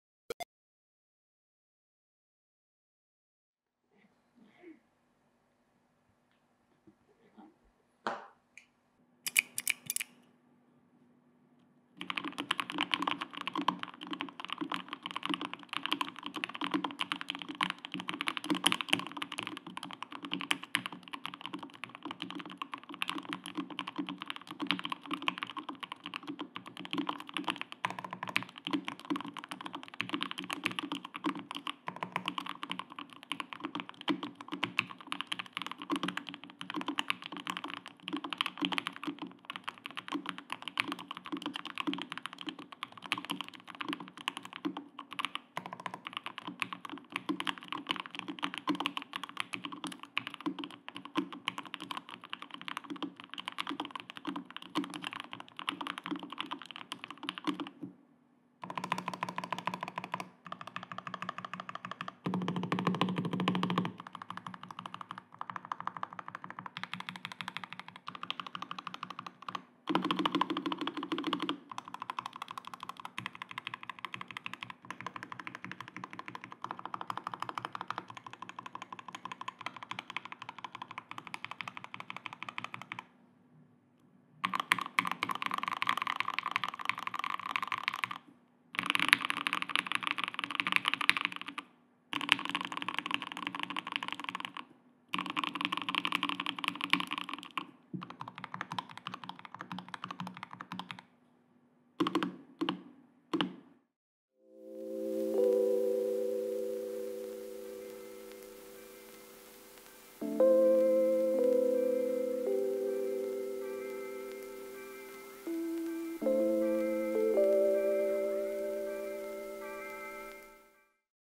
Thic Thock Marshmallow V1 Switches - w/J-02 LE Keyboards Typing Sounds
Thic Thock Marshmallow Switches - w/J-02 Keyboards

KBD Build :
Keyboard : J-02
Plate : CF Plate
Switches : Thic Thock Marshmallow [ 205g0 ]
Spring : Cherry Gold Plate 65g [ 105g0 ]
Stabilizers : Cherry OG stabilizers [ 205g0 / Dielectric Grease]
Keycaps :  Gmk olivia + hihihi enter
Cable : Cablemod
Deskmat : A.kidformyesterday x Febust channel

Gear List :
Lens : Fujinon XF 18-55 mm. F2.8-4R OIS / XF 23mm. F2R / XF 35mm. F2R
Mic : Boya BY-MM1 / Behringer XM8500
Audio I/F : RODE Ai-1 + Klark Teknik CT 1
Lighting : Nicefoto

👨🏼‍🚀 ขอบคุณสำหรับทุกๆการติดตามนะครับ 👨🏼‍🚀